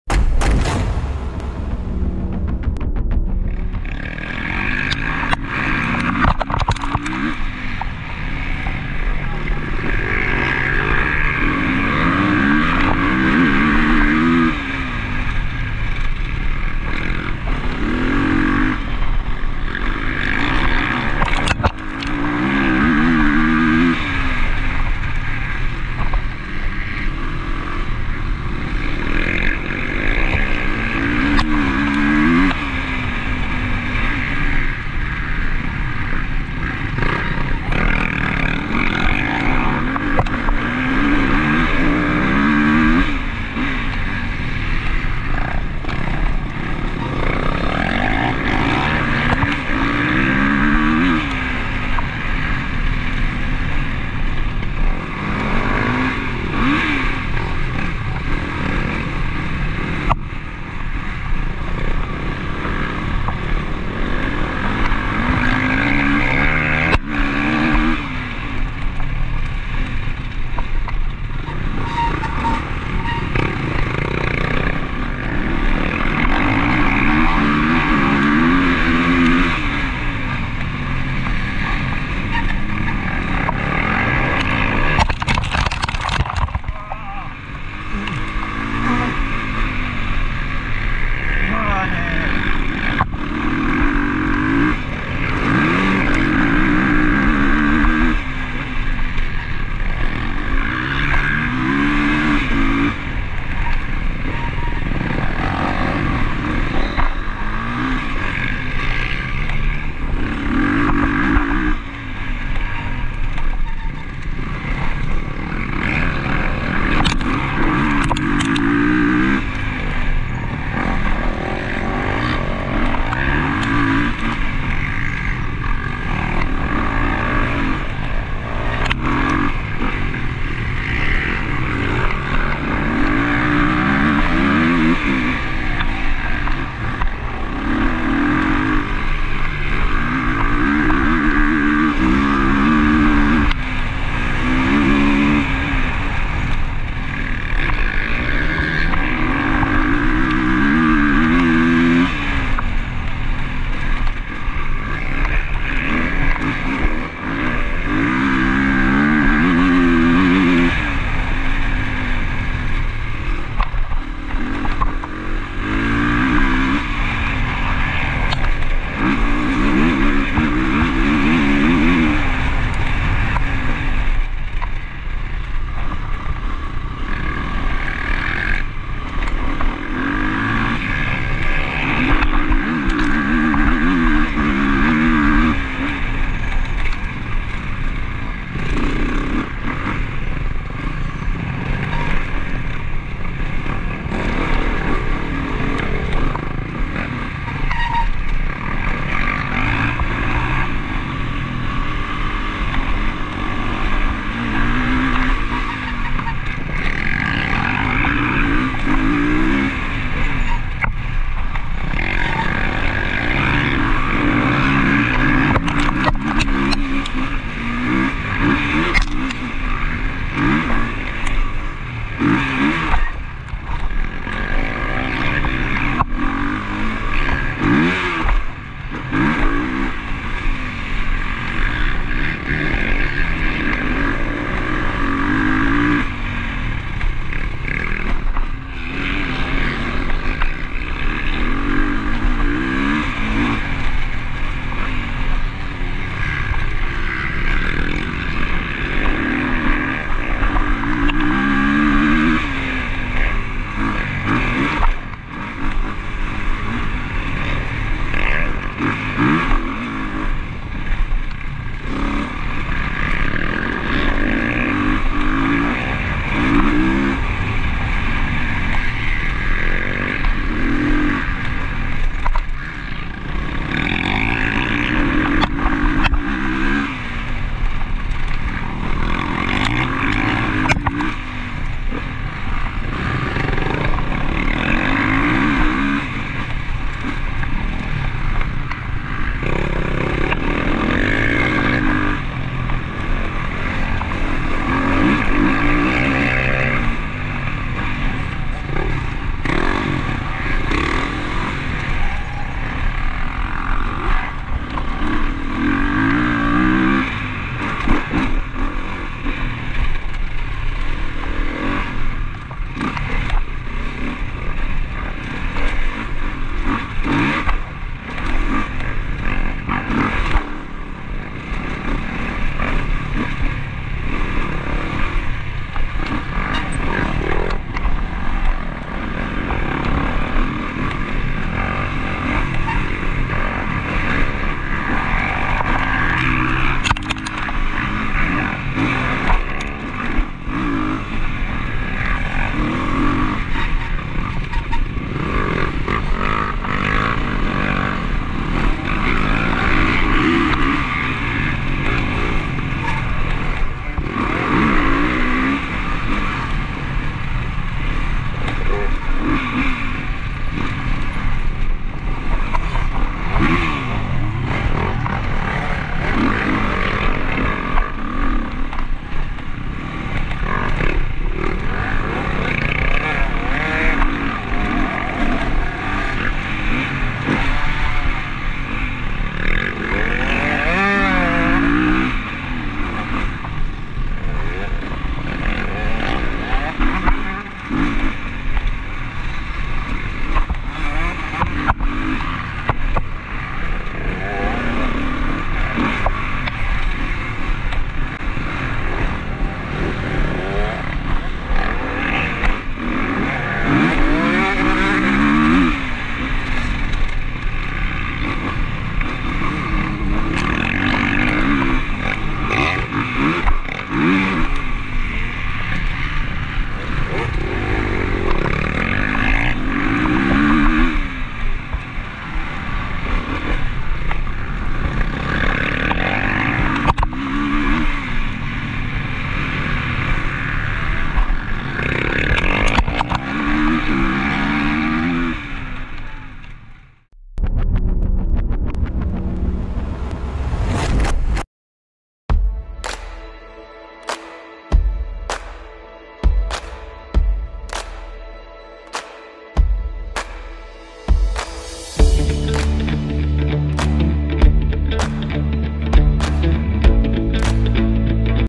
1 Runde in... | GCC Bühlertann 2023
Hey Leute.
Arvid hat sich mal wieder die GoPro umgeschnallt. Diesmal ist er beim Finallauf der GCC an den Start gerollt. Viel Spaß beim Anschauen!